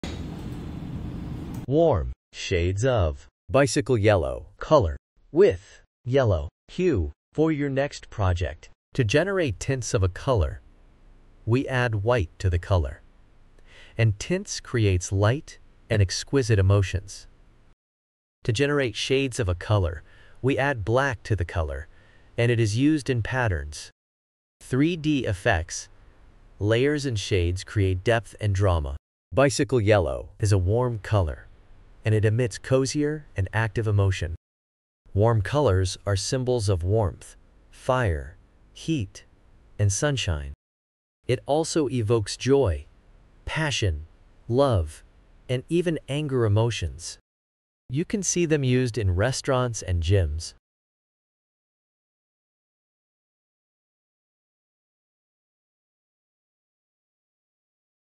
Shades & Tints of Bicycle Yellow color ffe58c A Warm Yellow color
Shades of Bicycle Yellow color ffe58c Warm Shades of Bicycle Yellow color with Yellow hue for your project!

Shades
Hex e2cb7c

Tints
Hex ffe798

Bicycle Yellow is a warm color, and it emits cozier and active emotion. Warm colors are symbols of warmth, fire, heat, and sunshine. It also evokes joy, passion, love, and even anger emotions. You can see them used in Restaurants and Gyms.